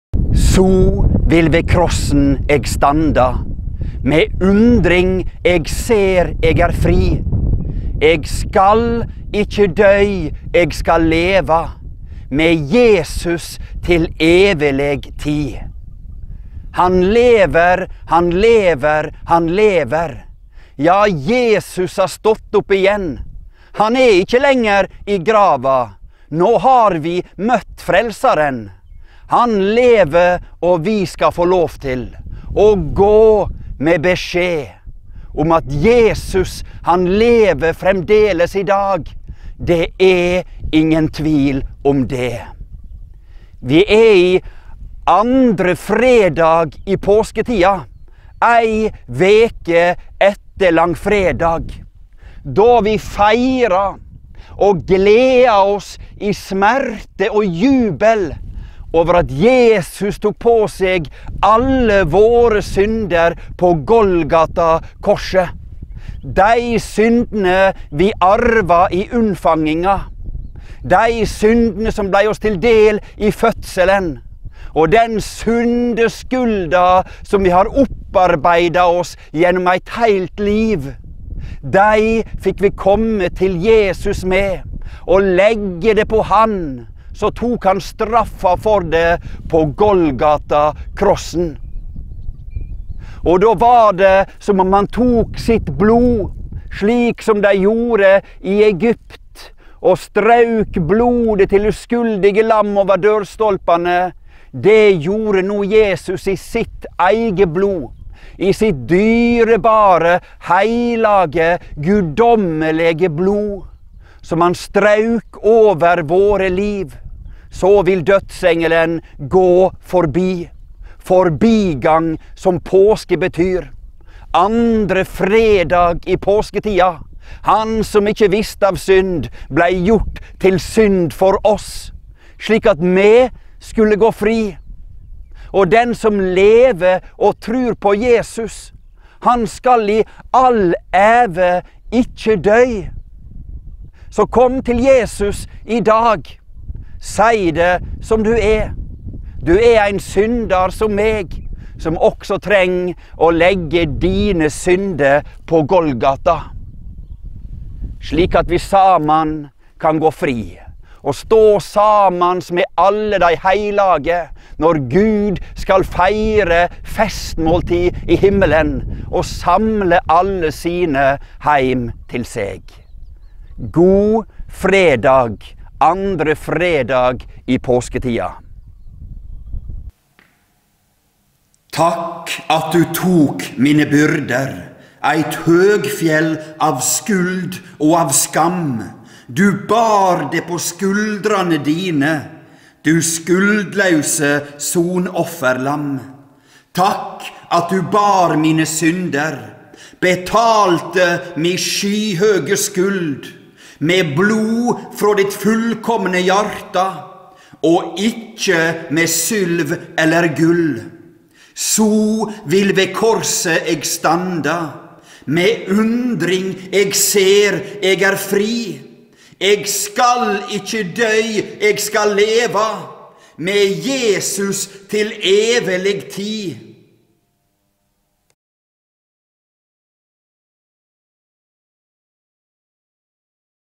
2. Fredag i Påsketida | Takk at du tok mine byrder | Kort andakt - Ei veke etter Langfredag.
2. Fredag i Påsketida. Påska 2020. Opptak ved Krossen på Berge. Finnøy - Noregs Kristne Hovudstad. Ei viktig påminning. Eit bodskap som ikkje går ut på dato. Uansett kva som skjer i verda, kva som råkar oss, er det viktig å komme til Jesus med sitt liv, med si synd, det som gjekk galt, og alt vi har med oss frå før.